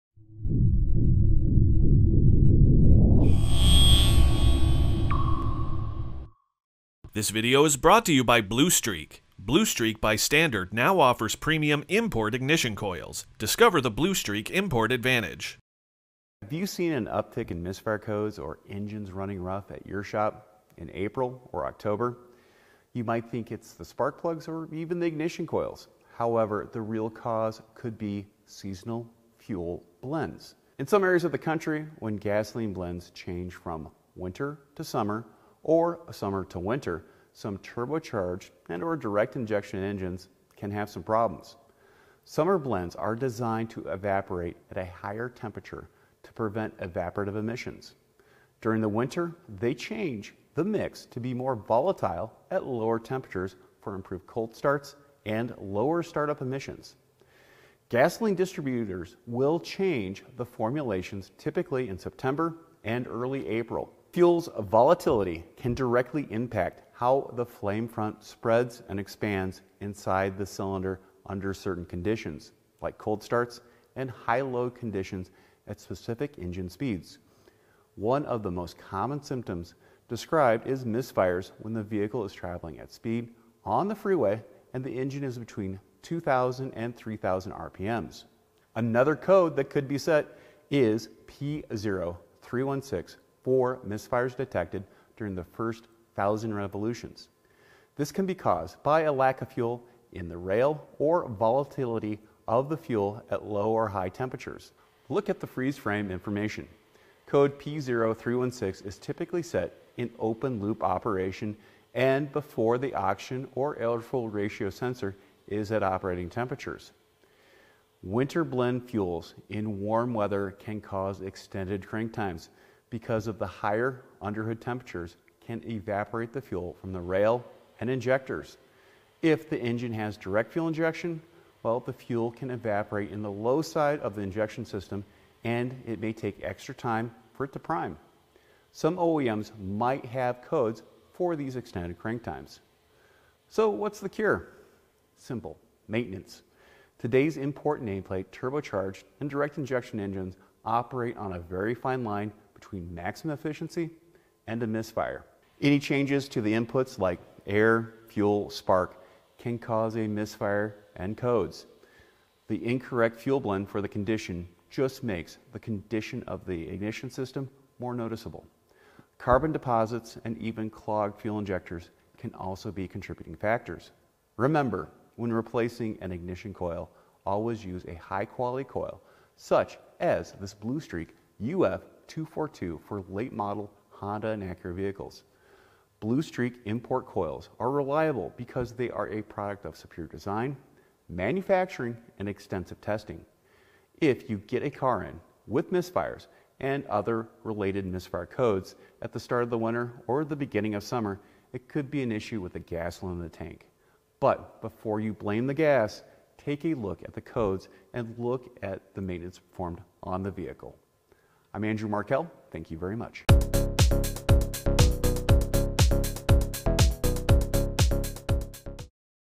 Seasonal Fuel Blends
An increase in misfire codes or rough running engines can be caused by seasonal fuel blends.